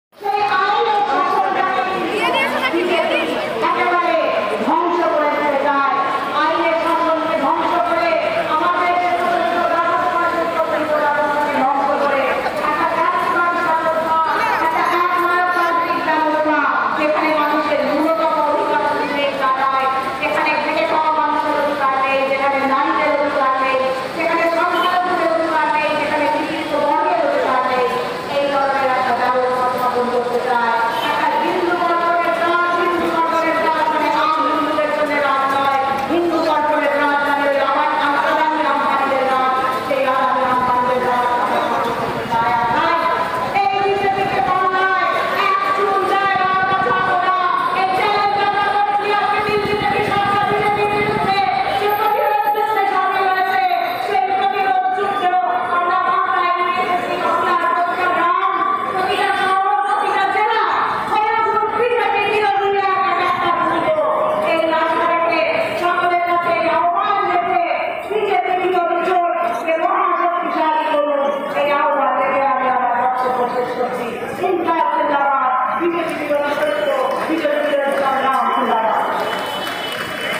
ফ্যাসিস্ট আর এস এস, বিজেপির বিরদ্ধে বাংলার মঞ্চে বক্তব্য রাখছেন শর্মিষ্ঠা চৌধুরী
কলকাতায় ১০ই মার্চ ফ্যাসিস্ট আর এস এস ও বিজেপির বিরদ্ধে বাংলার জনসভায় বক্তব্য রাখছেন শর্মিষ্ঠা চৌধুরী